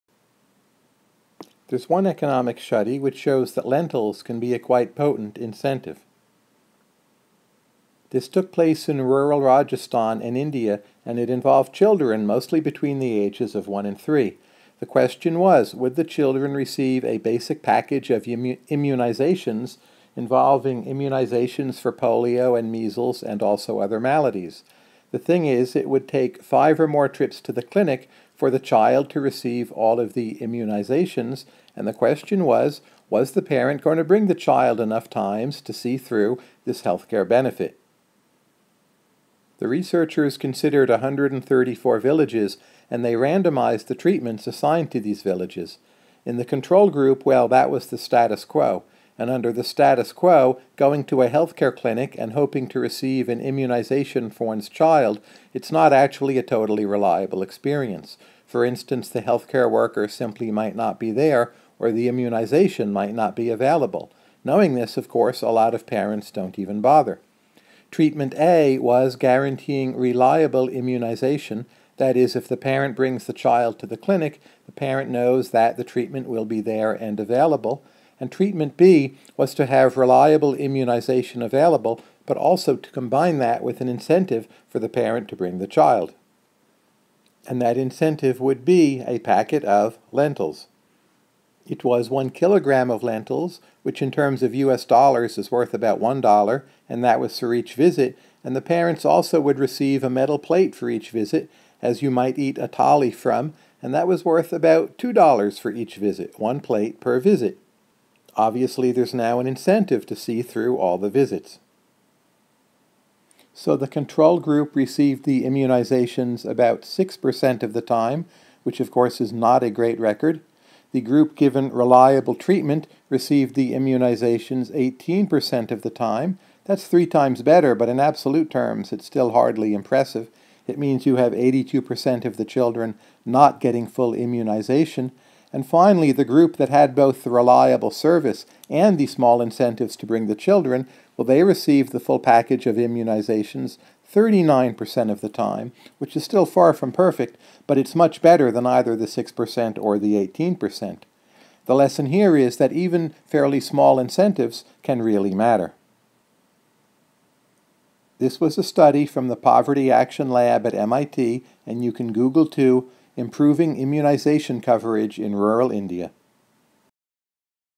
Lentils as incentive
One of the recurring themes of recent RCTs is that apparently small incentives can make a big, big difference.